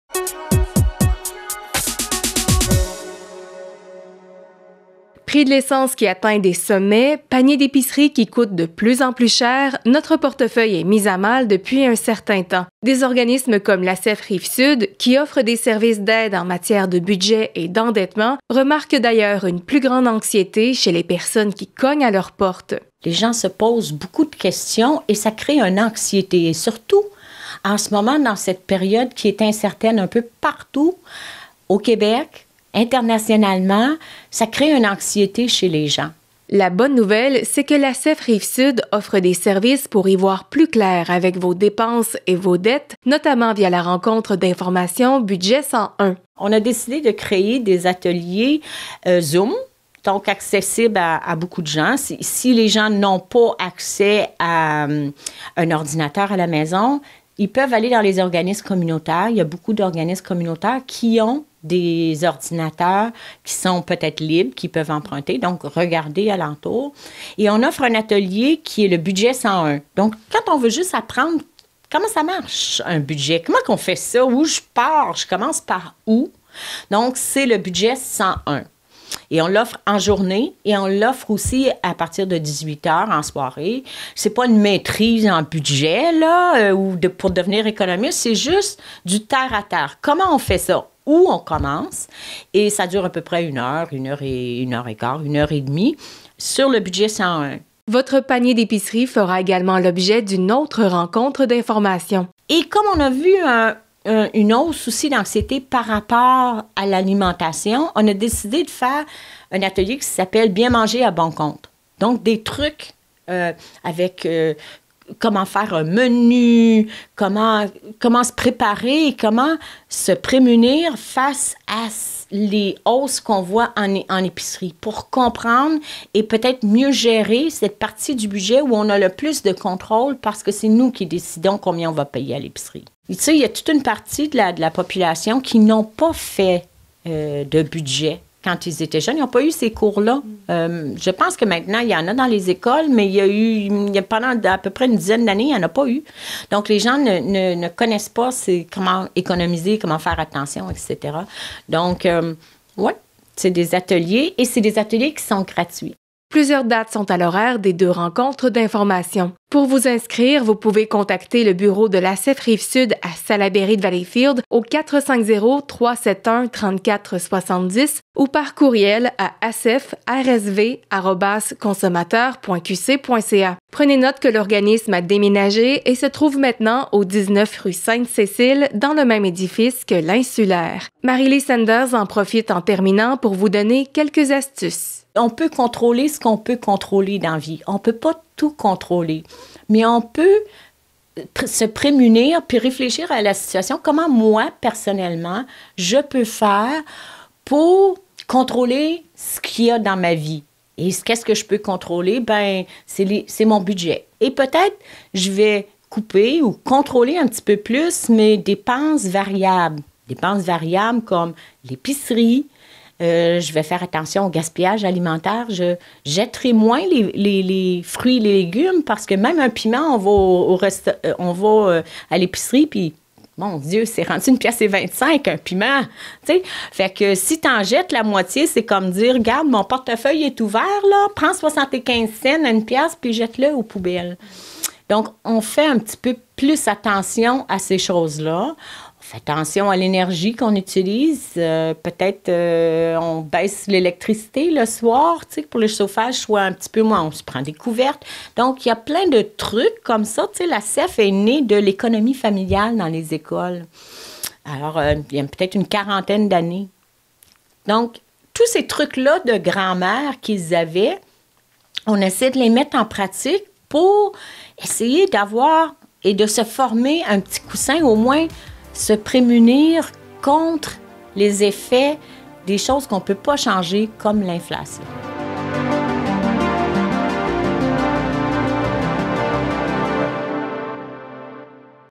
Connecté Valleyfield - Deux ateliers en ligne pour économiser
L'ACEF Rive-Sud de MTL vous propose deux ateliers en ligne pour économiser : "Bien manger à bon compte" et "Budget 101".